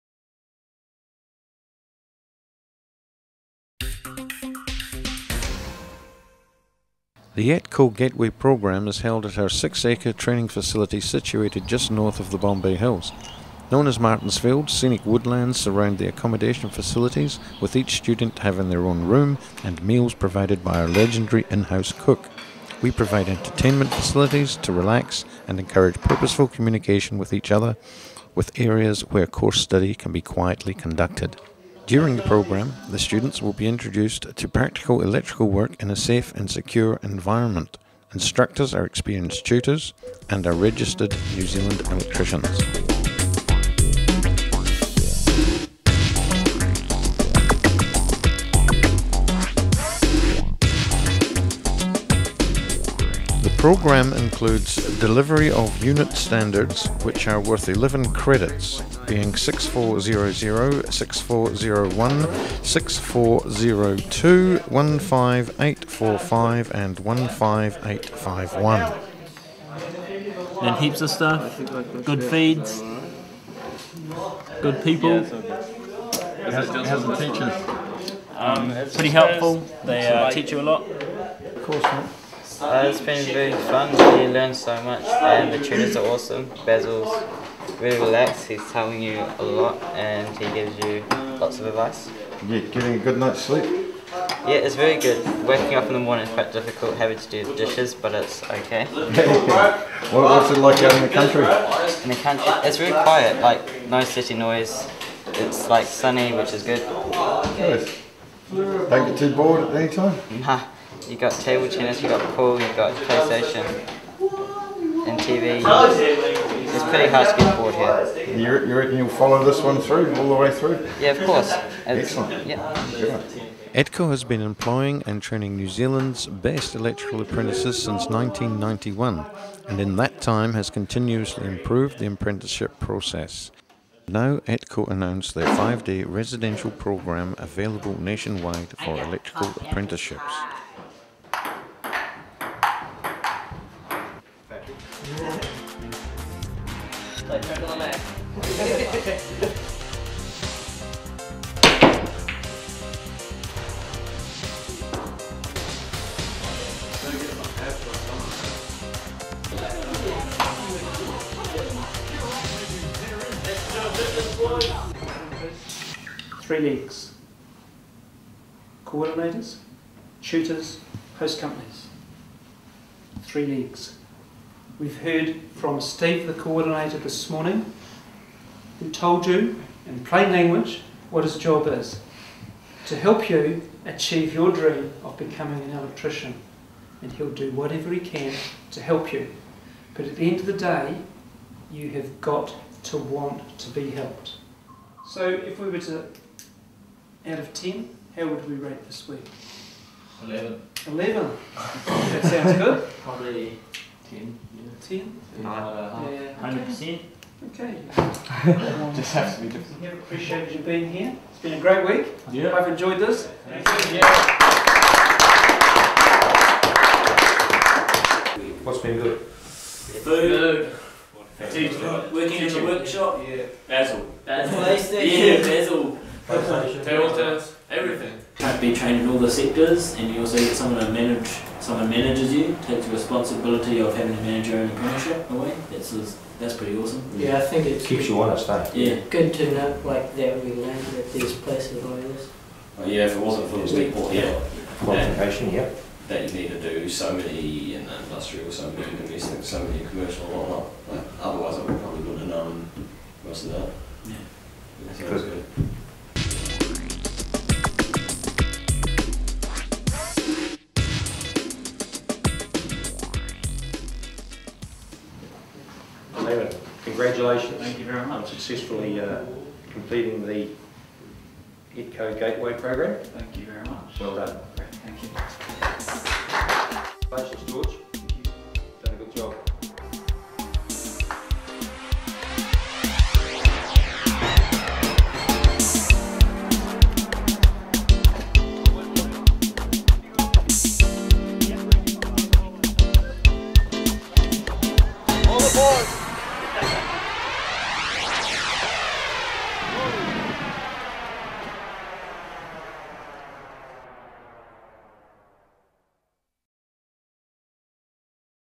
Etco Gateway Course for Electrical Apprenticeships
Course designed to introduce school students to the Electrical trade as offered by ETCO New Zealand in accordance with the Education Dept. "gateway" Initiative